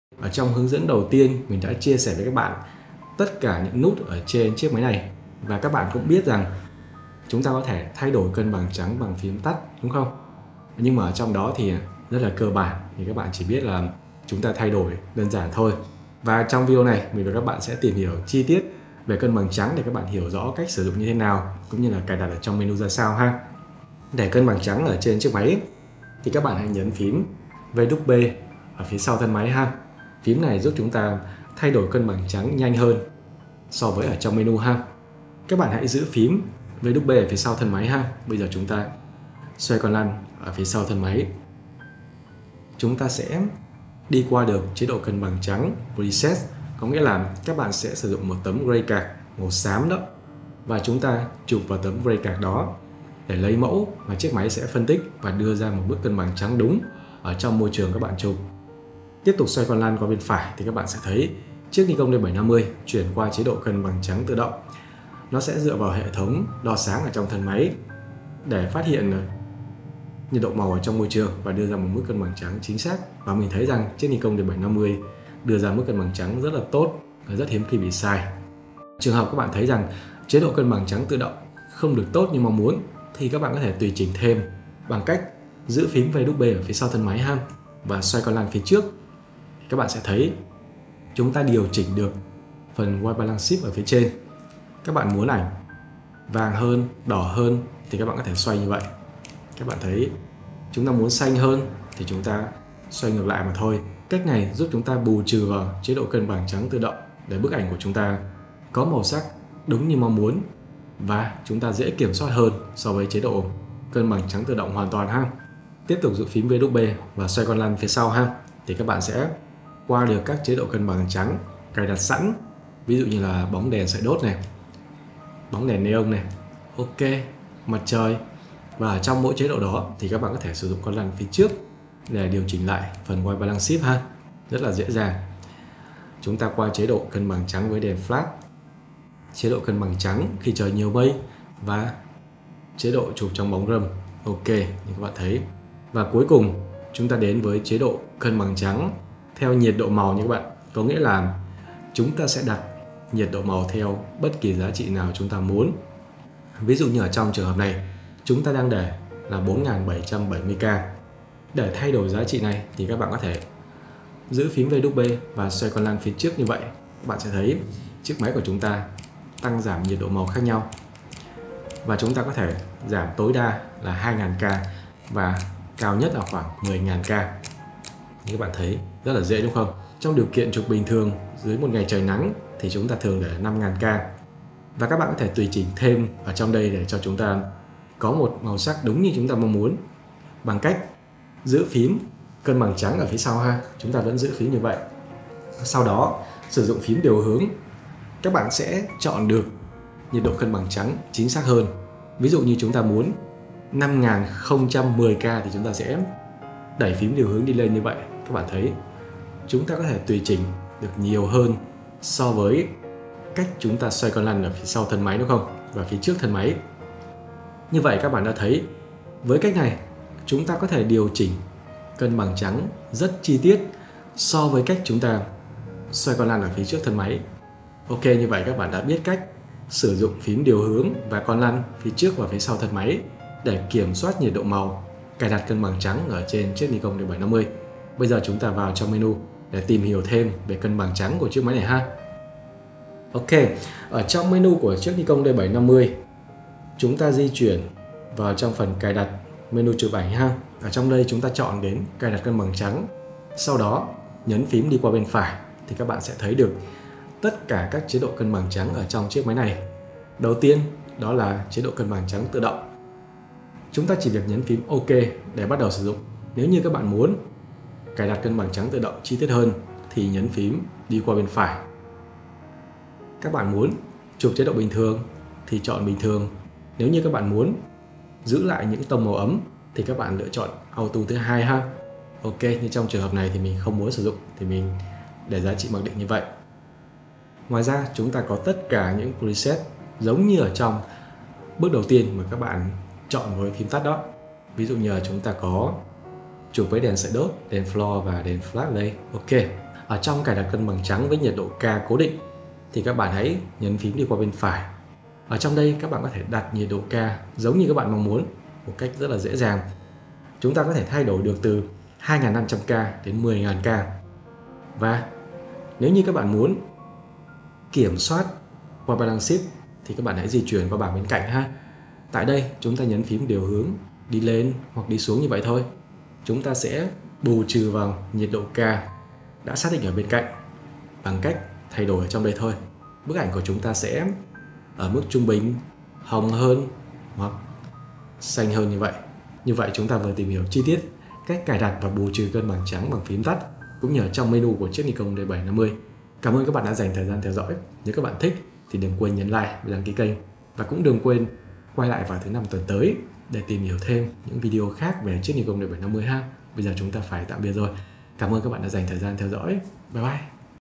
Cài đặt cân bằng trắng trên Nikon D750
Cách cài đặt cân bằng trắng trên Nikon D750. Giúp bạn biết cách điều chỉnh cân bằng trắng bằng phím tắt. Ngoài ra, bạn cũng biết cách chỉnh White Blance Shift trong Menu.